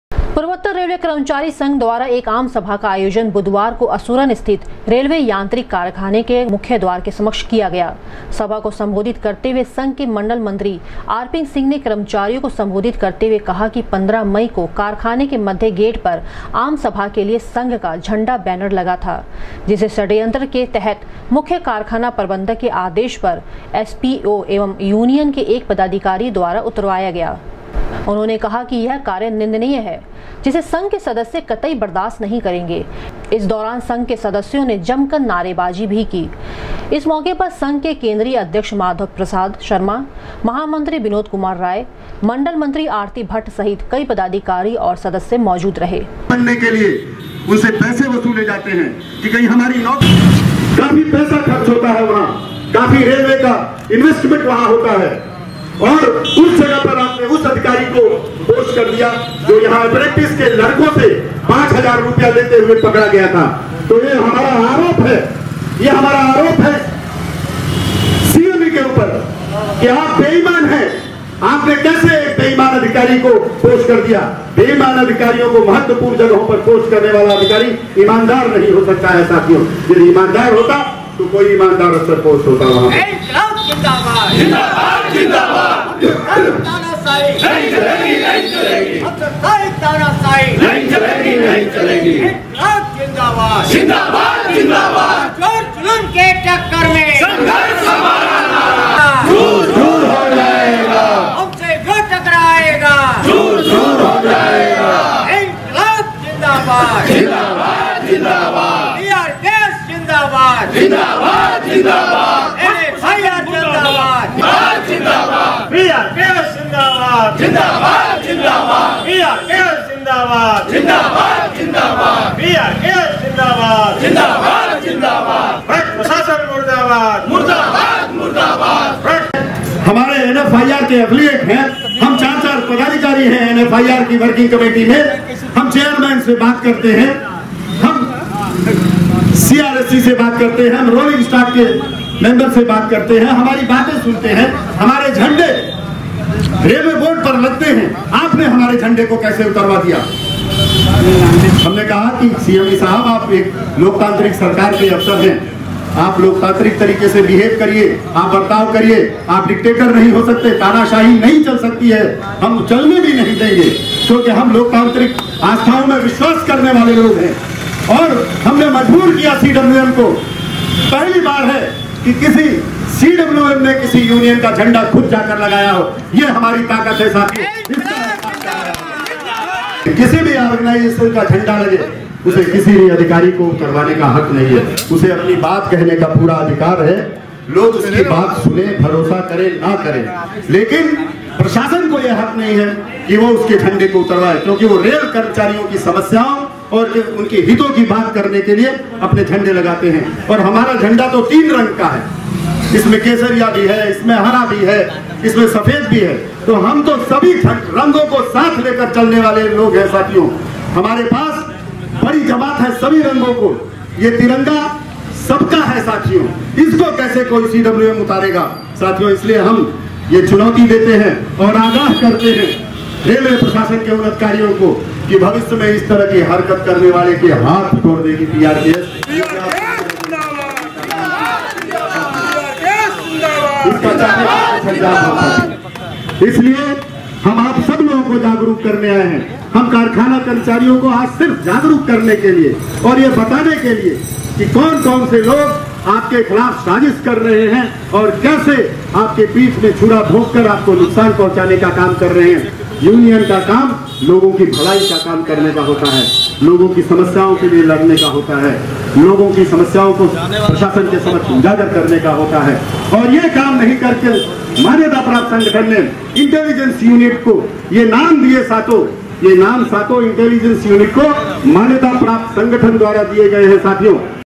पूर्वोत्तर रेलवे कर्मचारी संघ द्वारा रेलवे यांत्रिक कारखाना के समक्ष आयोजित हुआ आम सभा
पूर्वोत्तर रेलवे कर्मचारी संघ द्वारा रेलवे यांत्रिक कारखाना के समक्ष आयोजित हुआ आम सभा, संघ के झंडे उतारे जाने पर सदस्यों ने व्यक्त किया आक्रोश